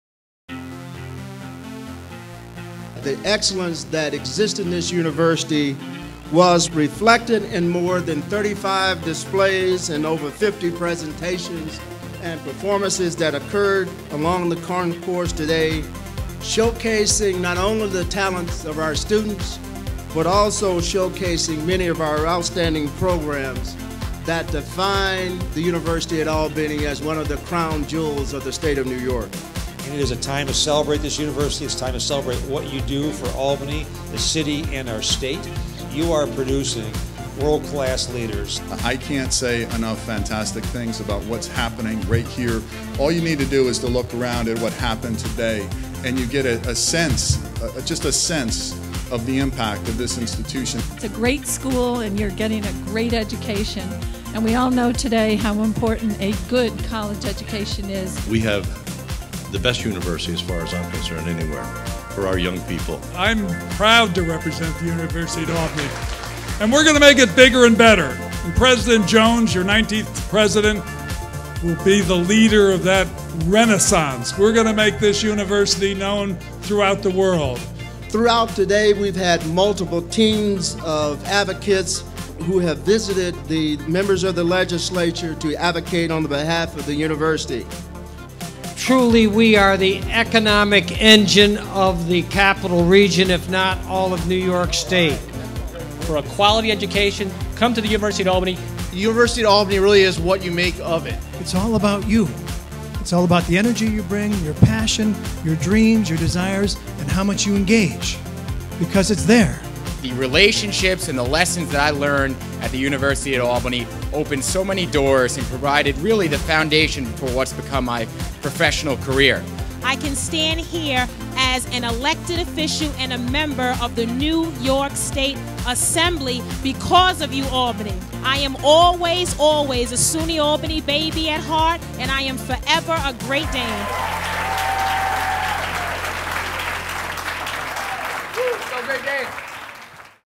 UAlbany Day at the New York State Capitol!
We showed why Great Danes are great at the annual UAlbany Day event at  the New York State Capitol.